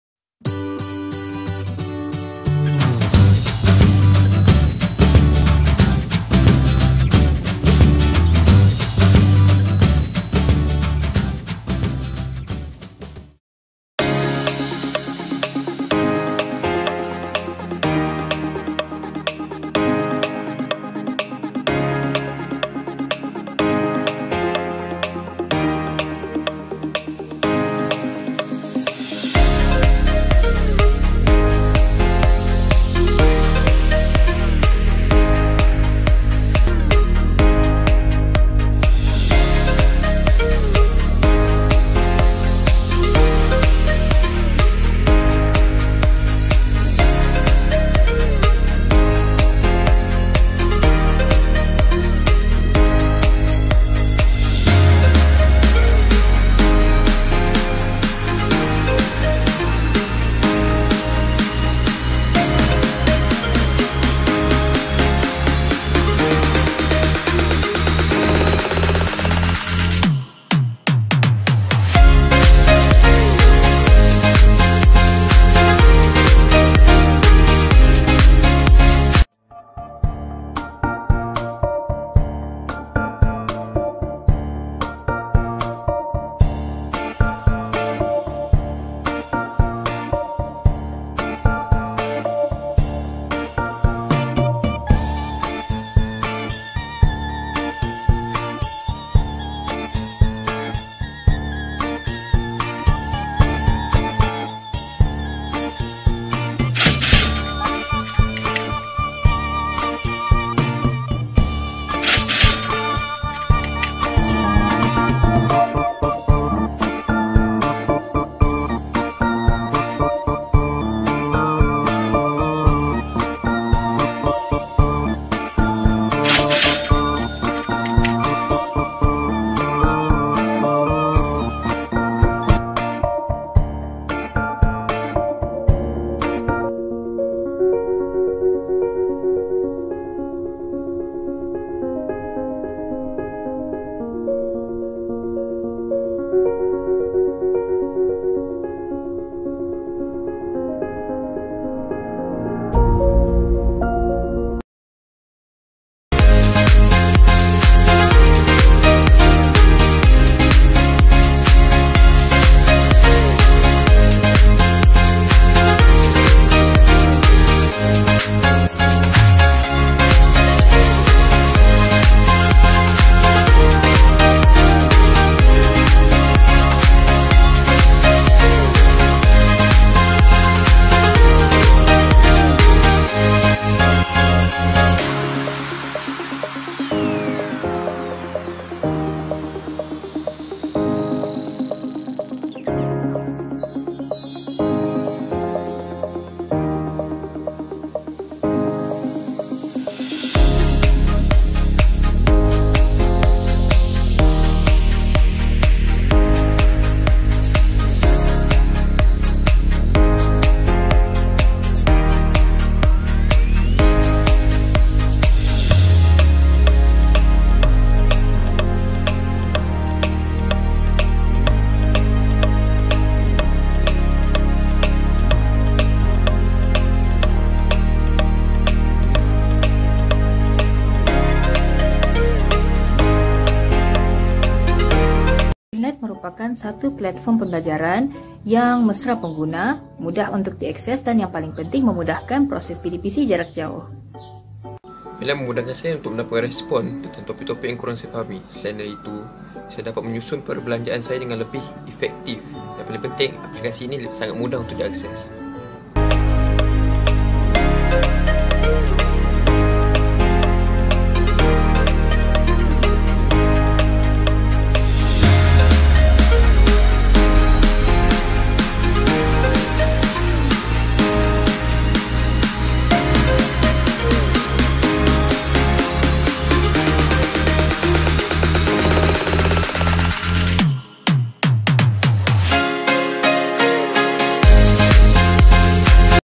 FINLITE: Financial Literacy
FINLITE is the first educational mobile apps in the market consists of financial literacy element incorporated with economics curriculum. This innovation have won Gold Medal in IUCEL 2021.

Inventors:
AP Dr. Rohaila Yusof
AP Dr. Zainizam Zakariya
Prof Dr. Stanley Yap Peng Lok
AP Dr. Mohd Asri Mohd Noor